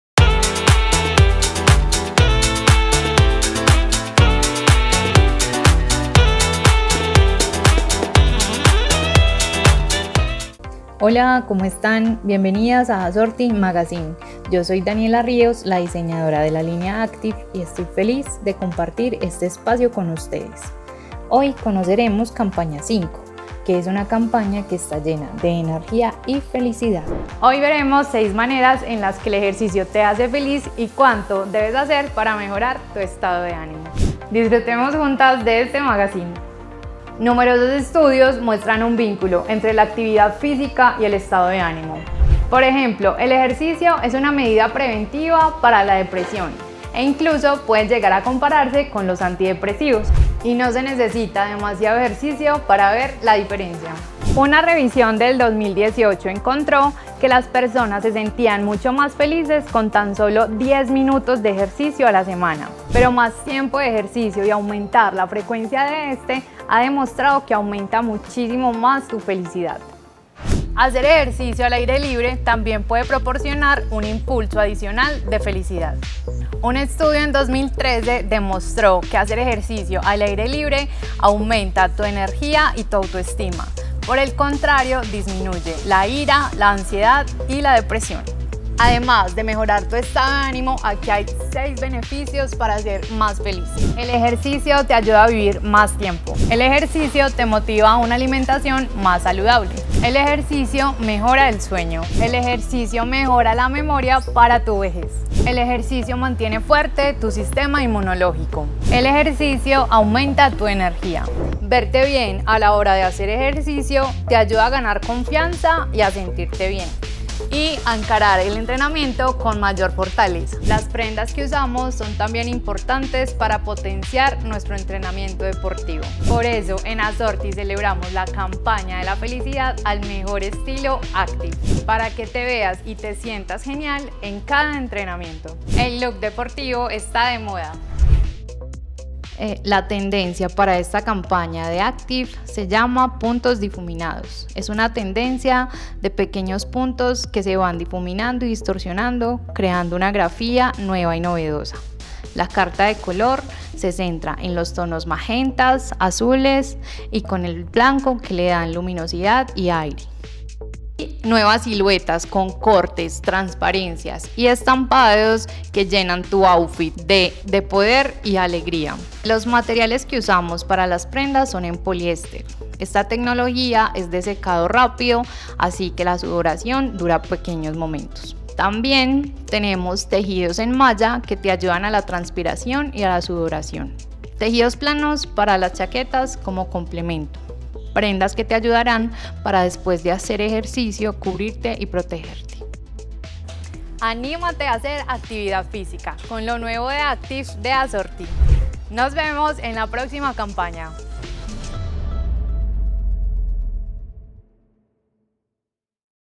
Sensaciones más acogedoras y reconfortantes - Magazine de moda C5
Esta campaña está llena de energía y felicidad, por eso queremos compartir contigo 6 maneras en las que puedes ser feliz haciendo ejercicio.

Las prendas que usas al momento de entrenar te ayudan a potenciar tus ganas de ejercitarte, sintiendo mayor confianza mientras te ves genial con un estilo más deportivo. Queremos celebrar contigo y nuestra línea Active, la campaña de la felicidad.

Esta nueva colección llega cargada de una tendencia de estampados de puntos con magnéticos colores magentas y azules, dándole un aire de majestuosidad, poder y alegría a cada prenda. Esta línea tiene tecnología de secado rápido haciendo que el sudor no dure mucho, protegiéndote durante cada entrenamiento.